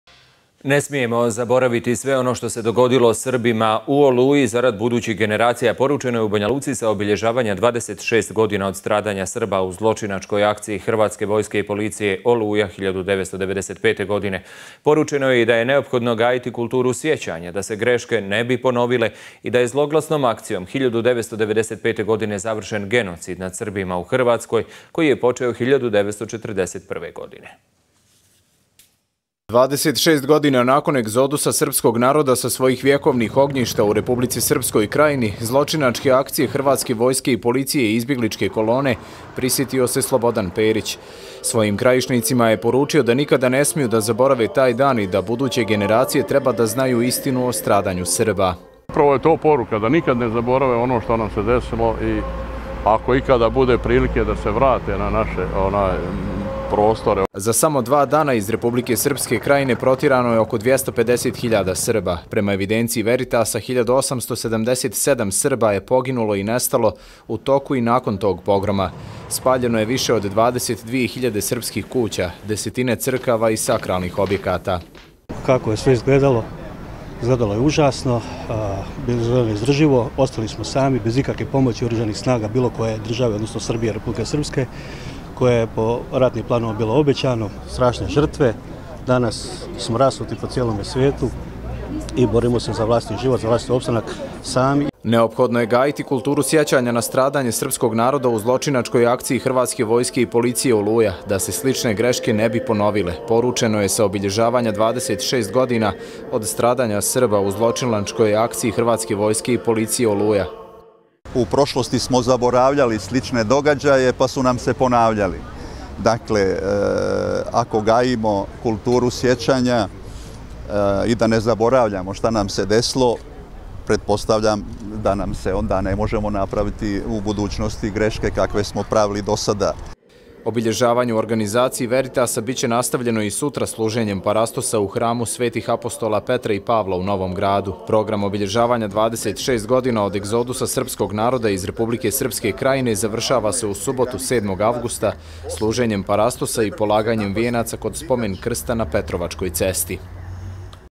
Štrbac: Izjava medijima povodom obeležavanja 26. godina stradanja Srba u Oluji, Banjaluka, 5.8.2021.
Direktor Dokumentaciono-informacionog centra „Veritas“ Savo Štrbac poručio je danas da je neophodno gajiti kulturu sjećanja na stradanje srpskog naroda u zločinačkoj akciji hrvatske vojske i policije „Oluja“ da se slične greške ne bi ponovile.